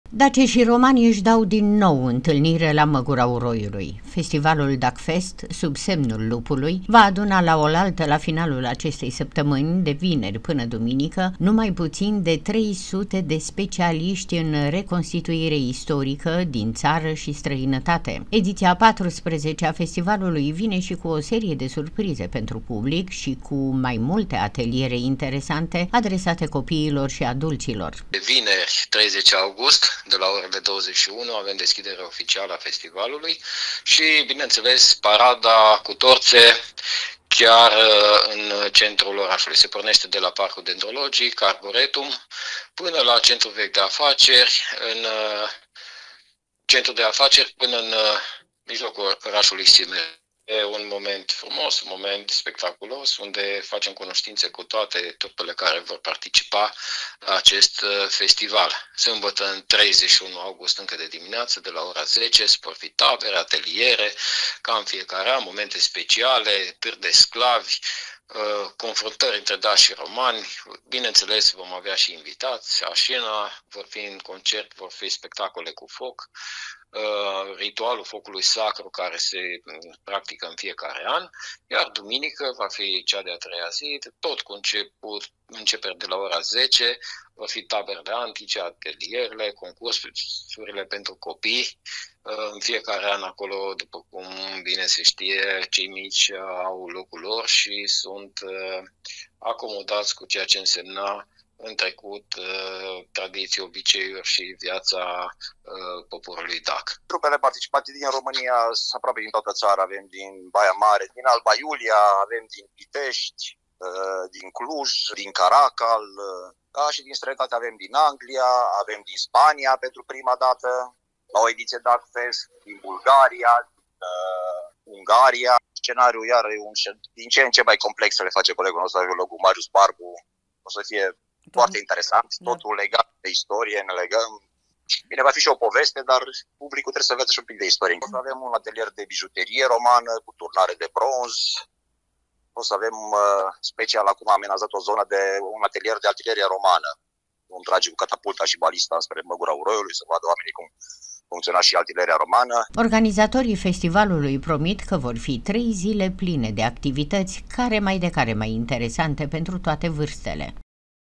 DACII ȘI ROMANII ÎȘI DAU, DIN NOU, ÎNTÂLNIRE LA MĂGURA UROIULUI
LA FESTIVALUL DACFEST „SUB SEMNUL LUPULUI” VOR PARTICIPA 300 DE REENACTORI DIN ȚARĂ ȘI STRĂINĂTATE